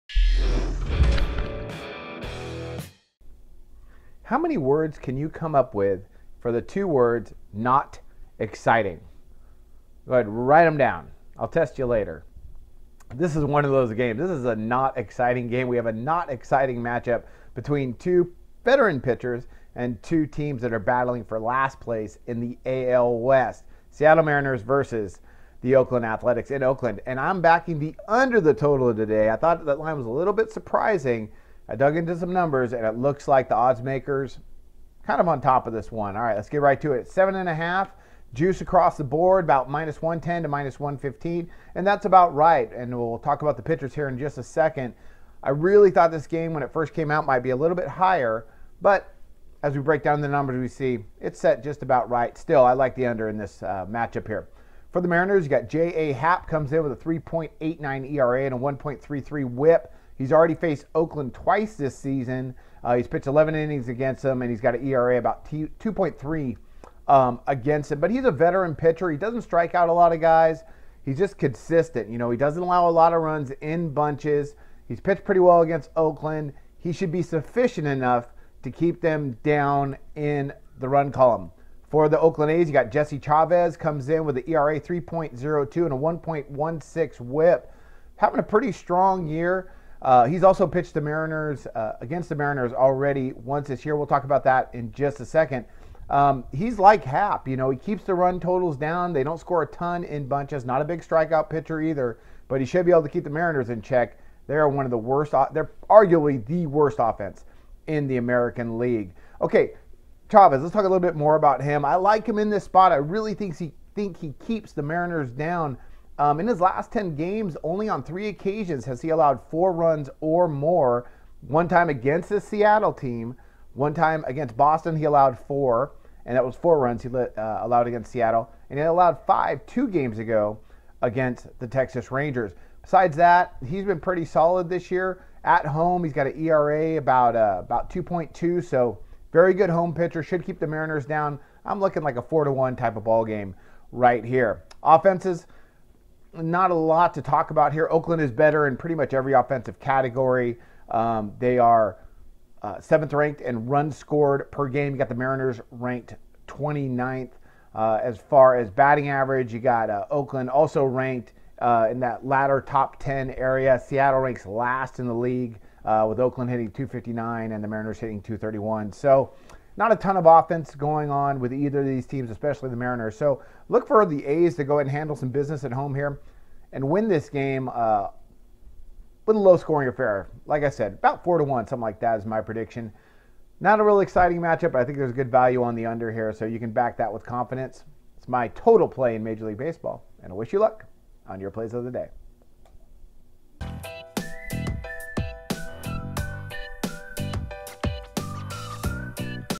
Seattle Mariners vs Oakland Athletics | MLB Picks | Low Scoring Affair
Expect a low scoring game today at the O.co Coliseum between Seattle Marinera and Oakland Athletics. Expert handicapper Charles shares his picks and explain why to take the 'Under'.